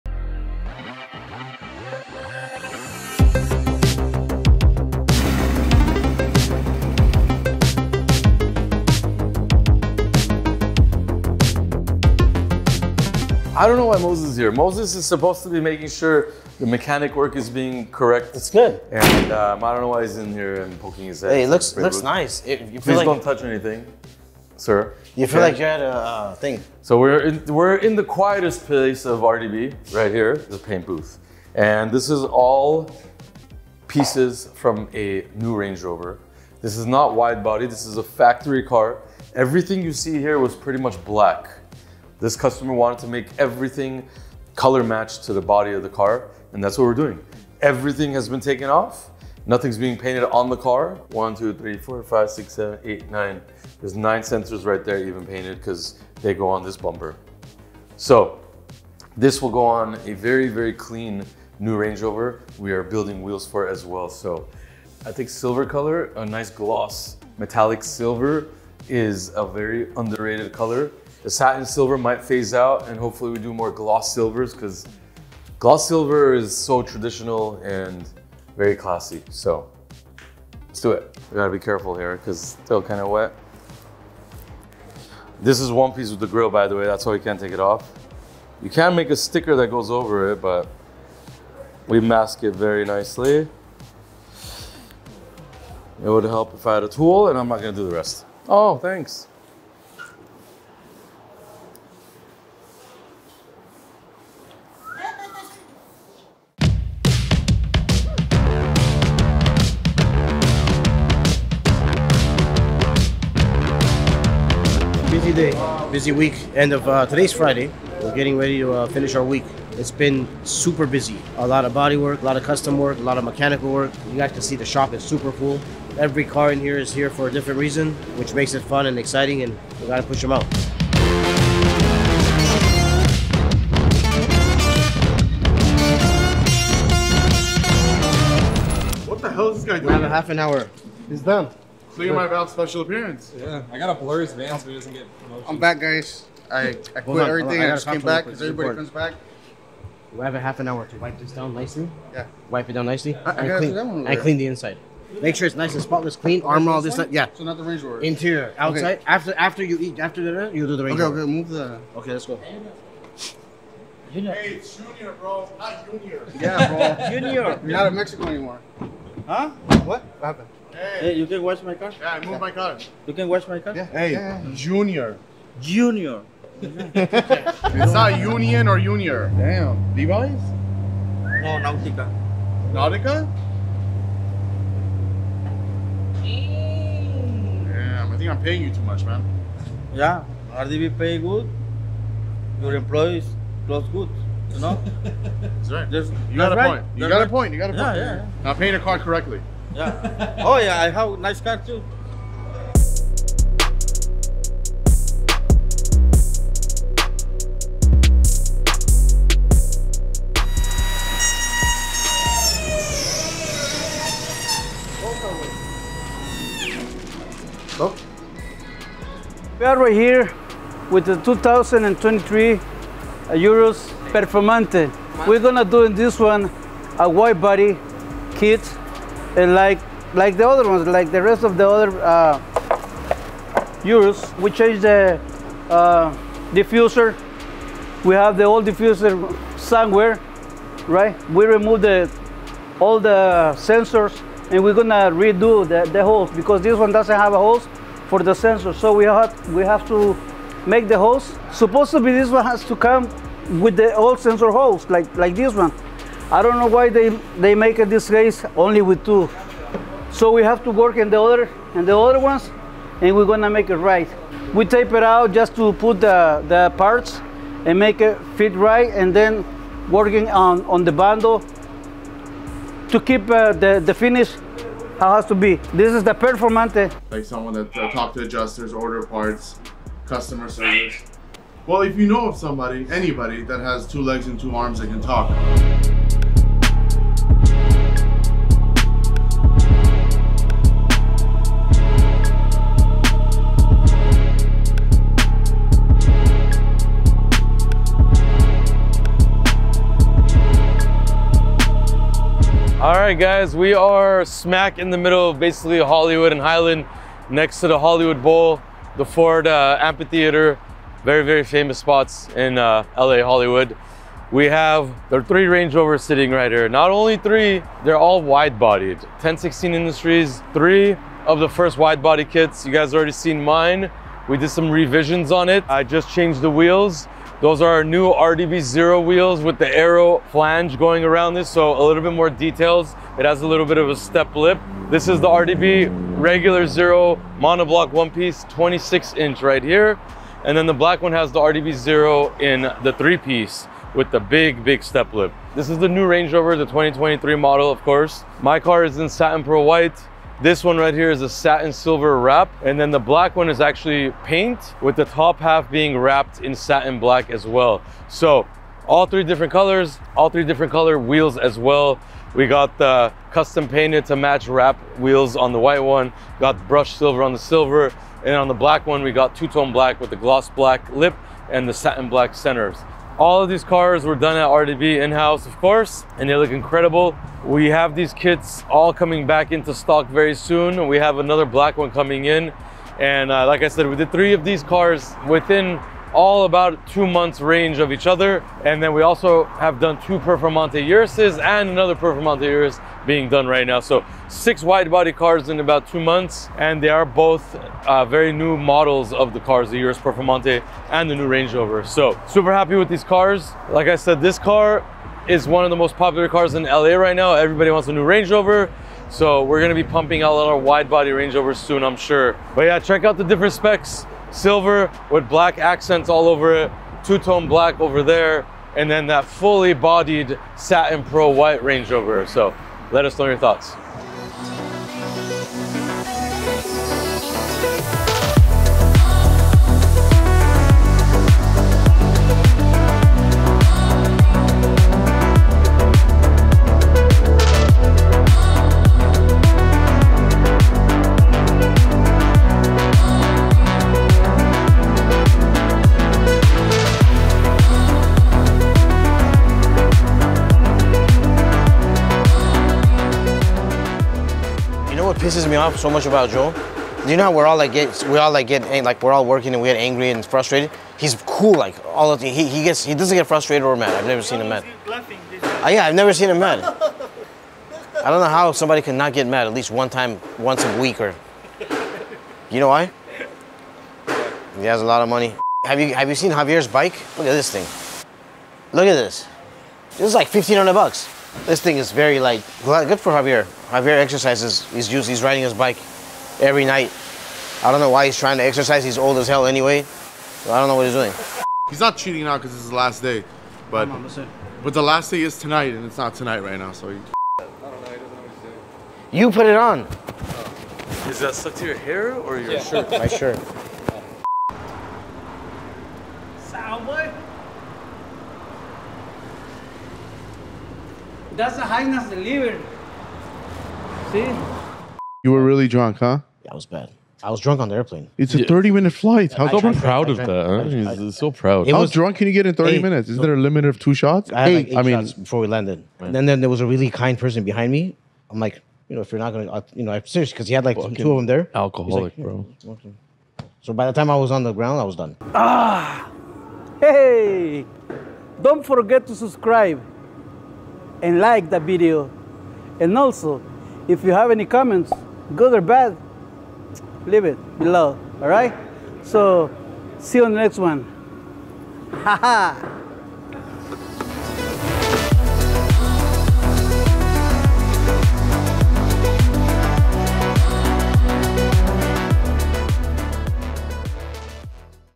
3 INSANE Widebody Range Rover's in Los Angeles.
Heart of Los Angeles with 3 Brand New Widebody Range Rover's! All built by RDB in house WITH 1016 Industries kits, featuring 3 different configurations of our famous RDB ZERO Wheel design! We are pumping out widebody projects every week!

ALSO NO PAID PROMOTIONS, NO FRILLS CAR SHOP.

INSANE SF90 Spider Ferrari with Full Body Kit, 1st in the World. + Green Porsche 992 Turbo S.